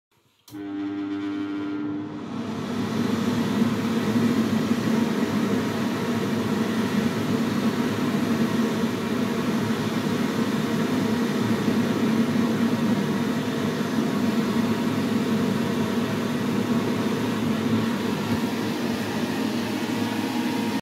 Unbranded Anda hand dryer, Fishbourne Roman Palace ♿️, Fishbourne, West Sussex
This hand dryer was not very powerful and didn't dry my hands that well, the air was warm and it was quiet, even though it had a bad start up sound.

This was located by the entrance, I haven't been here for over 14 years so I wouldn't remember the hand dryer, this one is possibly newer than the one in the Gents Toilets.